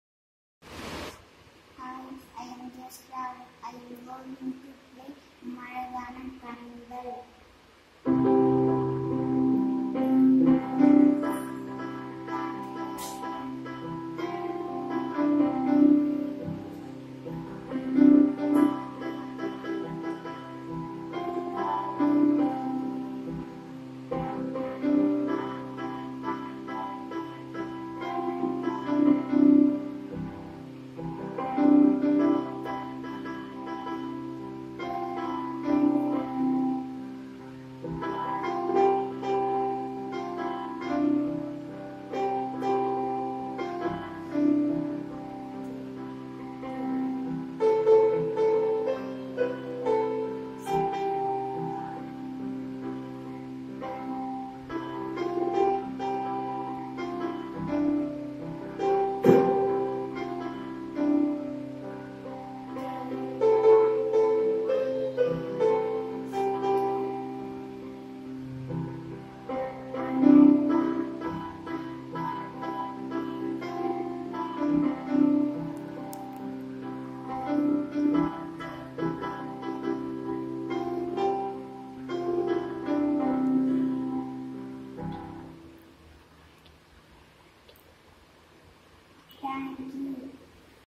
ROSARY MUSIC SCHOOL, THIYAGADURGAM, BY G.SWEETLIN JERSHA
Keyboard playing byG.jersha , S.V palayam.sankarapuram ,kallakurichi District